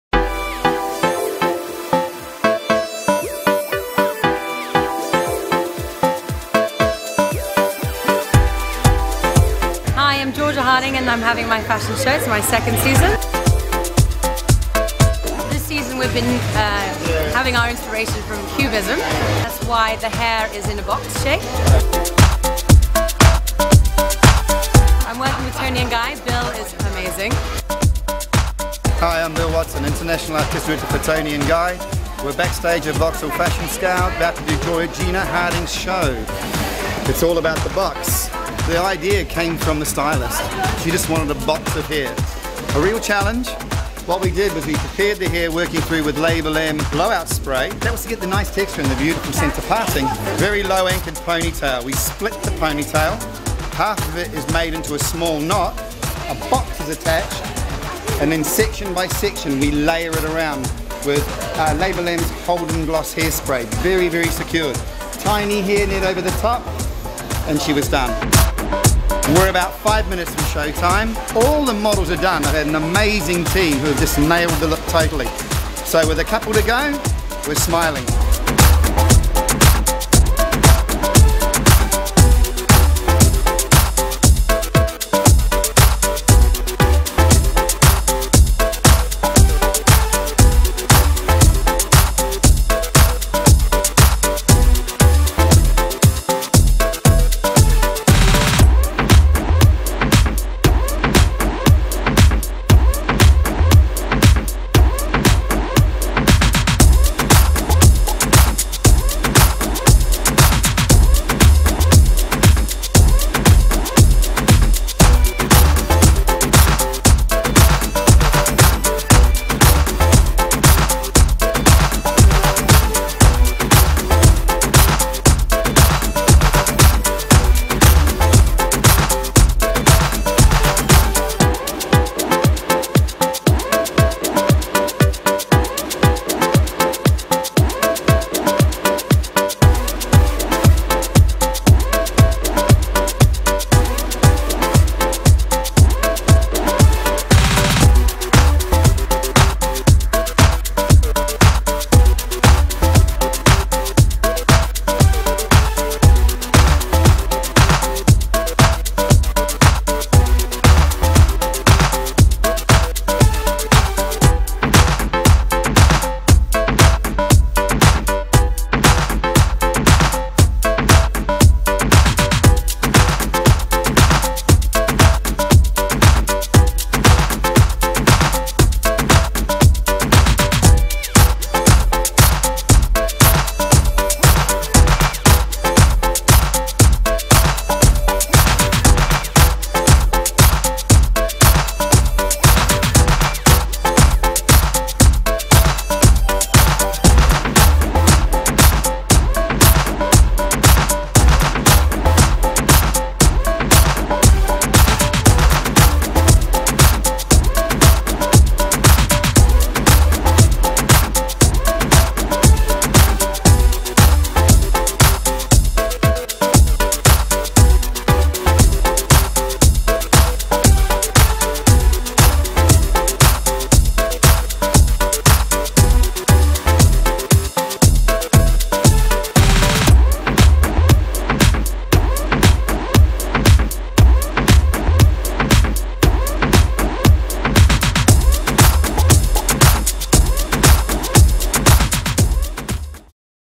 TONI&GUY @ Georgia Hardinge SS12 - London Fashion Week
Bill Watson heads up the TONI&GUY Session Team for the Georgia Hardinge spring/summer 2012 catwalk show. Here we bring you some exclusive backstage footage and show you how to achieve the runway hair looks at home. As the official sponsor of London Fashion Week, TONI&GUY consistently aims to bring key trends from the catwalk to the client.